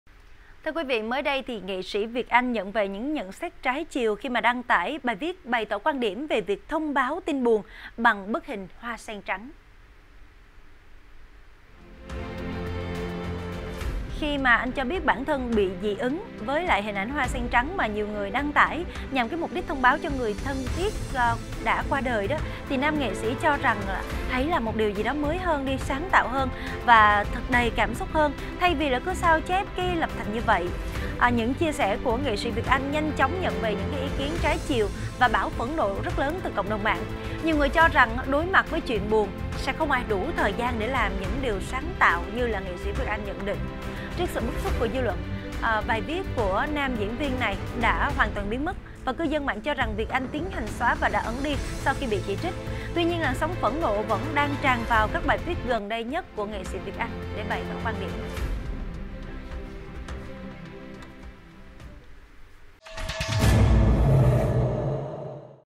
NS VIỆT ANH GÂY TRANH CÃI VÌ CHIA SẺ "DỊ ỨNG" KHI CÓ NGƯỜI THAY ẢNH HOA SEN TRẮNG | VTC9
VTC9 | NS VIỆT ANH GÂY TRANH CÃI VÌ CHIA SẺ "DỊ ỨNG" KHI CÓ NGƯỜI THAY ẢNH HOA SEN TRẮNG